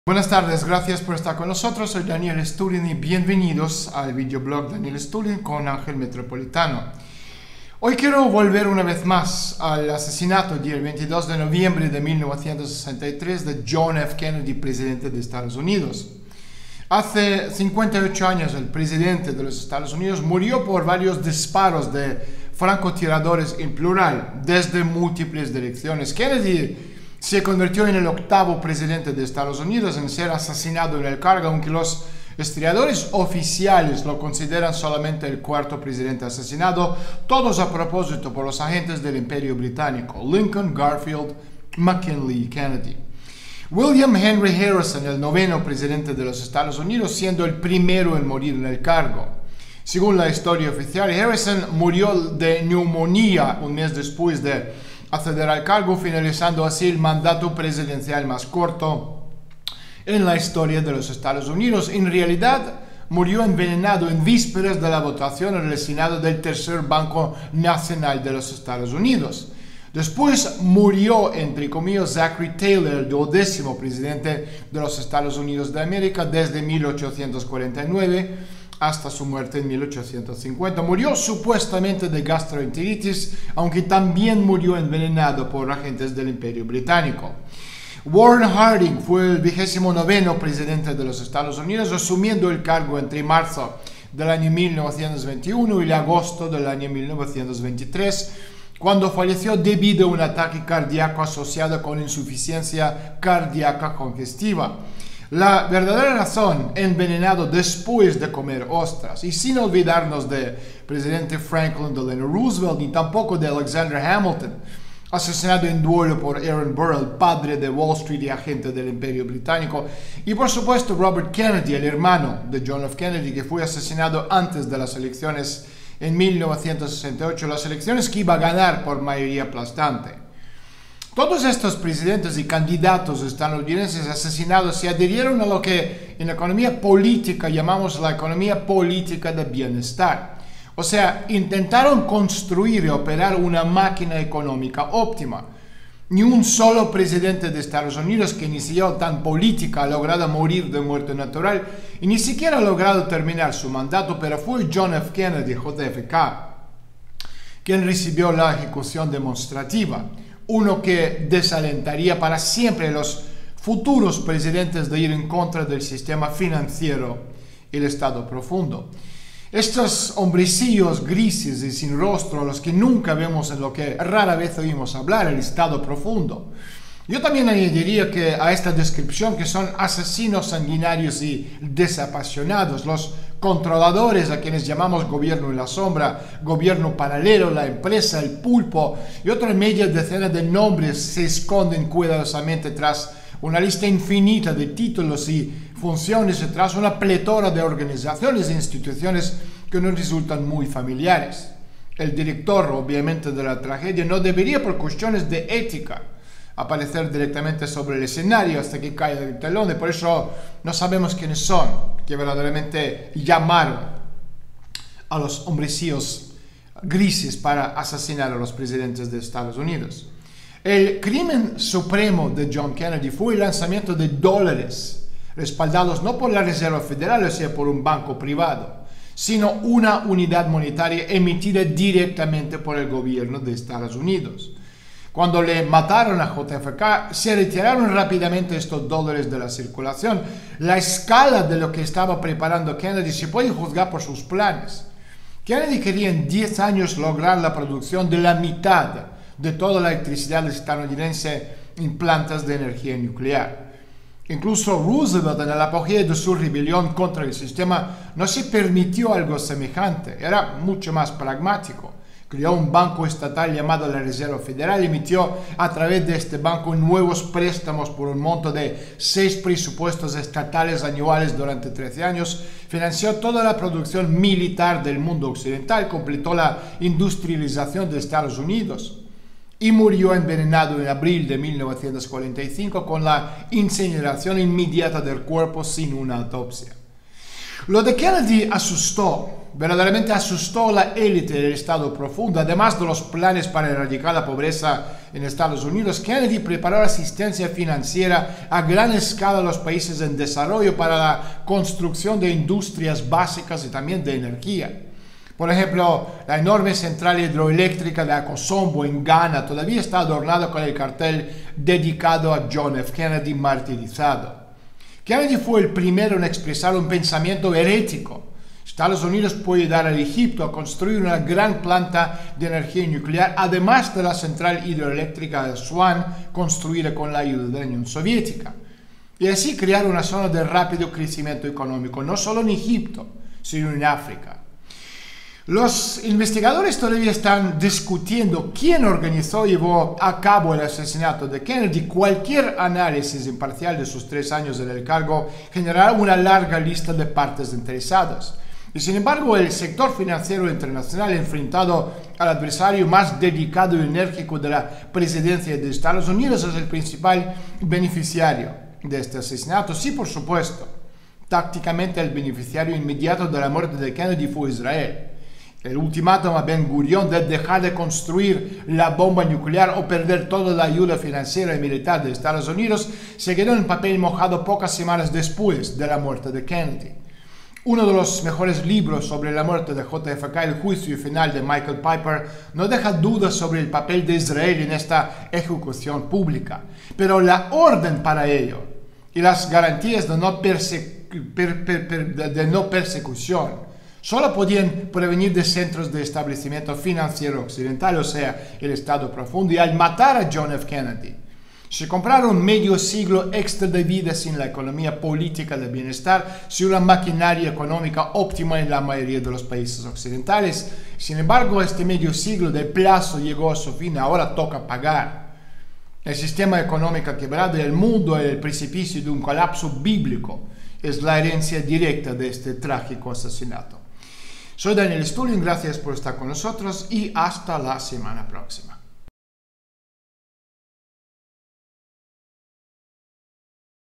Asesinato de John F. Kennedy | REVISITED | Daniel Estulin
Daniel Estulin sigue escarbando en las historias que cambiaron nuestras vidas para siempre, una de ellas, el asesinato de JFK, por qué y qué significó para el mundo de hoy, a continuación.